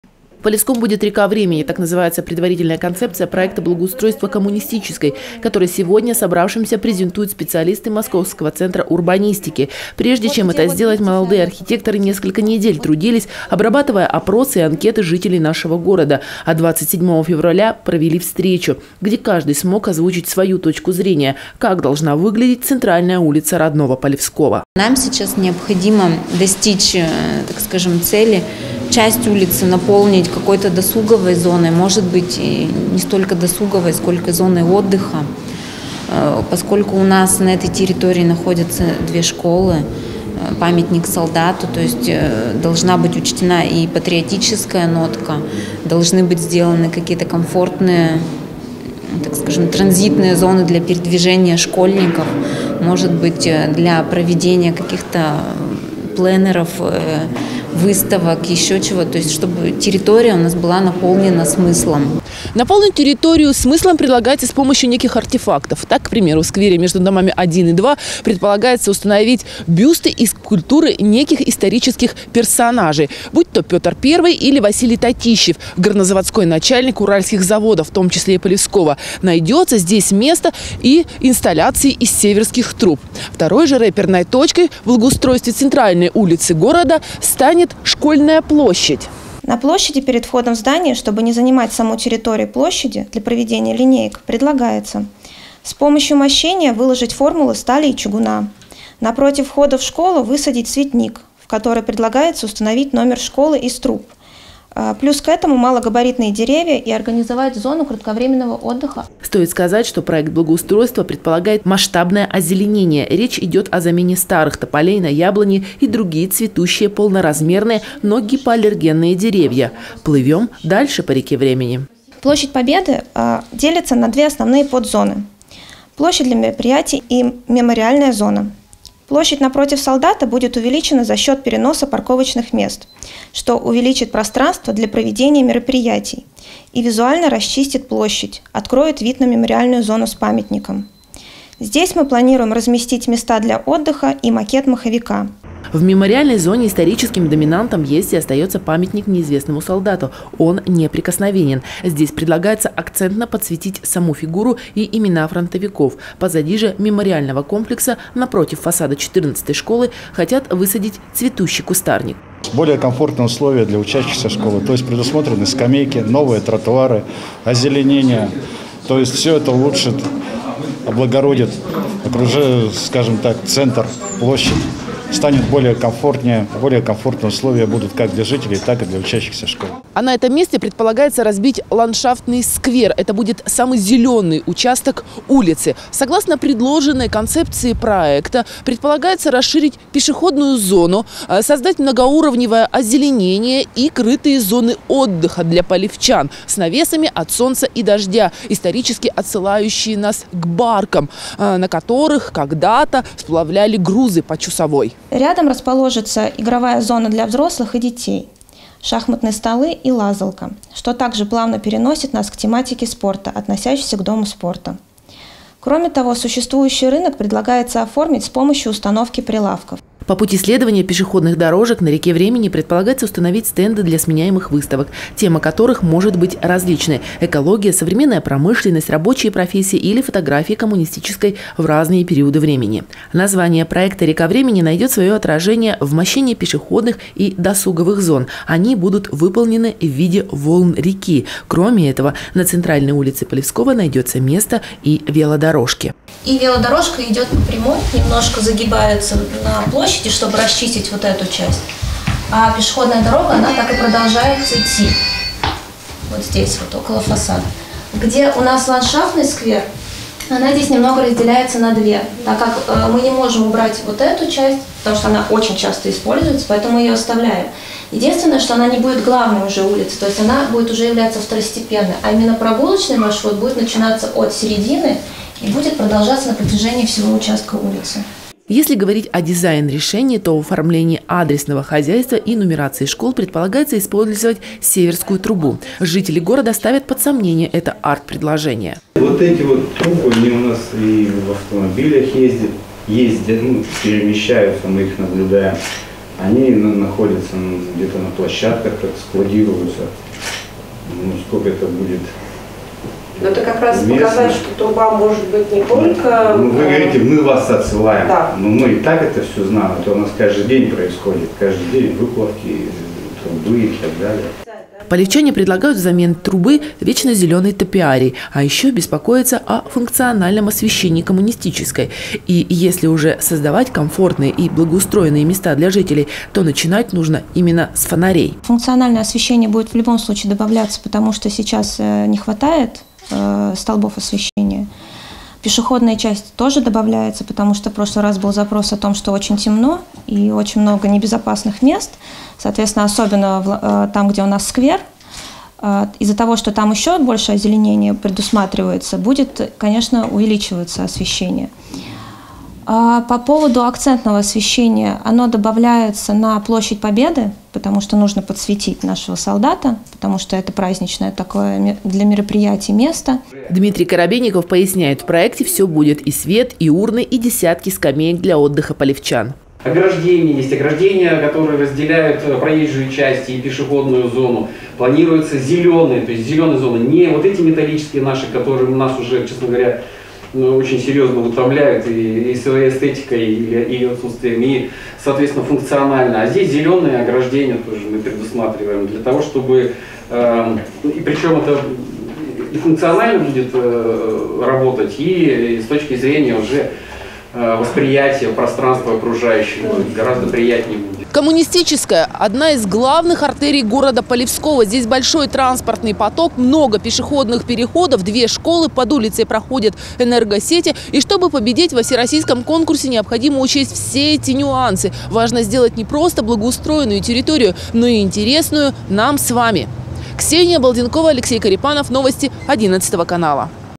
Полевская «Река времени»
Маховое колесо, ландшафтный сквер, мемориальная площадь, бюсты исторических персонажей и еще много других артефактов отсылающих нас к разным промежуткам в истории Полевского, могут появиться на Коммунистической, в случае победы нашей территории на Всероссийском конкурсе проектов по благоустройству. По задумке авторов центральная улица города станет рекой времени. Детально об этом проекте говорили накануне, мои коллеги все записывали и готовы рассказать об услышанном прямо сейчас.